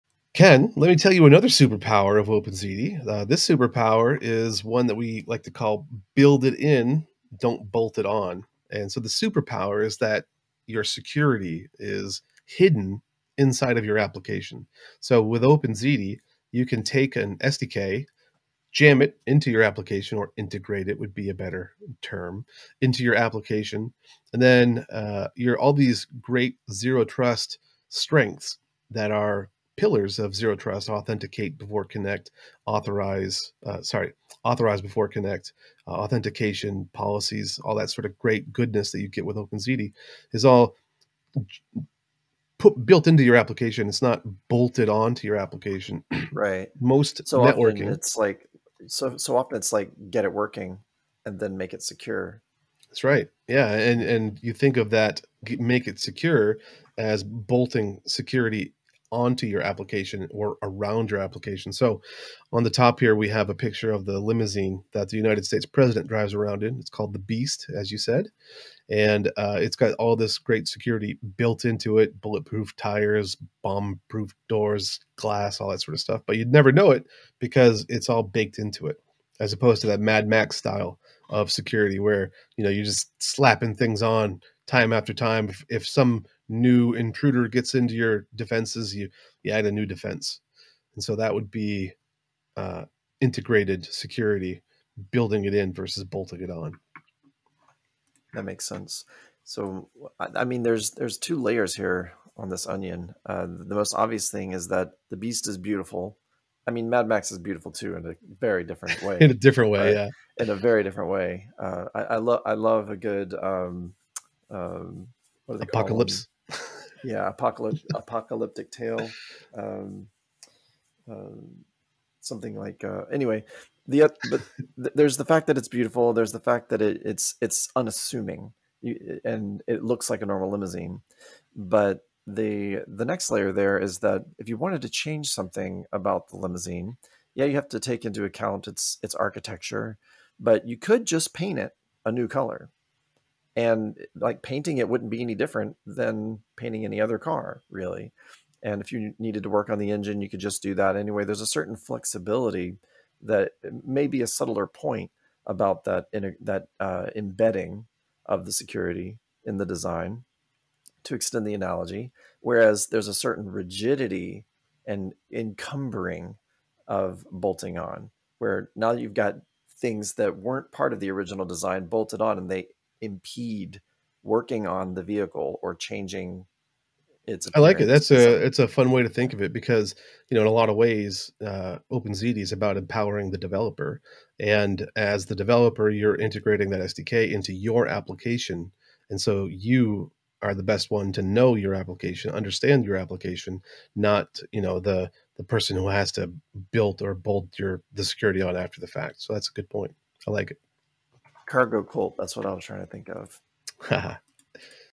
OpenZiti Superpower - Built in, not Bolted On
Build security *into* your application. Don't bolt it on after! Adopt an OpenZiti SDK, deploy an overlay network and build the security into your app from the start! Secure by default!